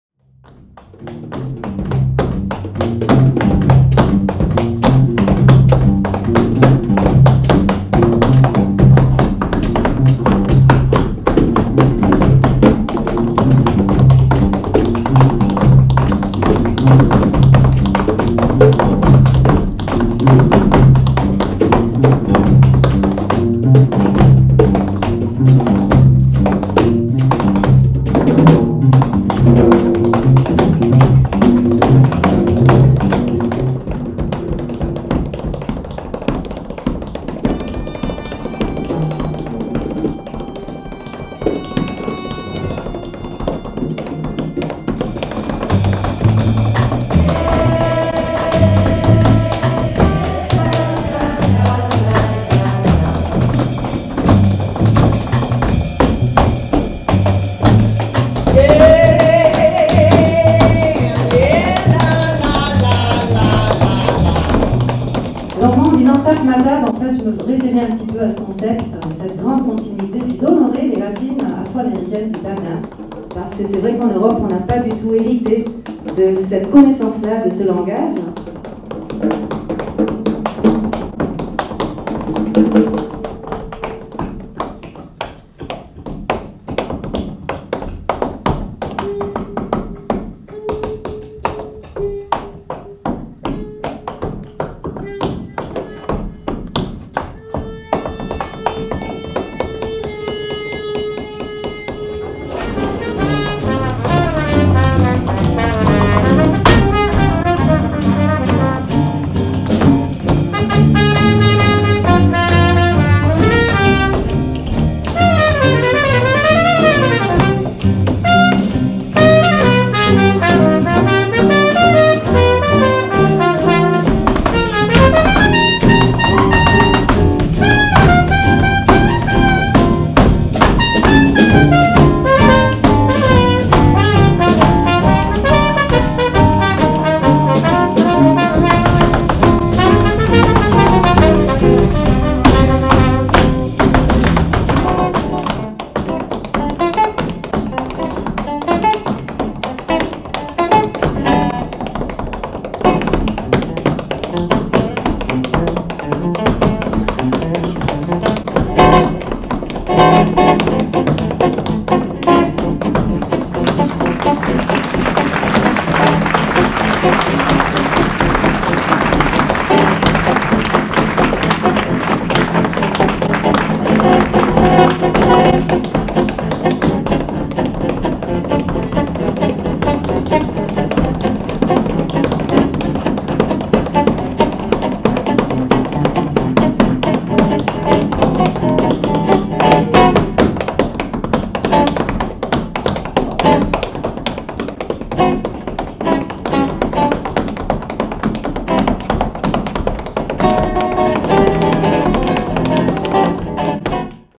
Roxane Butterfly's TAPmatazz - By way of Jazz
Teaser of Roxane Butterfly's TAPmatazz featuring a roster of versatile musicians and dancers who create a unique rhythmic landscape by blending expressive strength around the assertive power of the tap dance essence.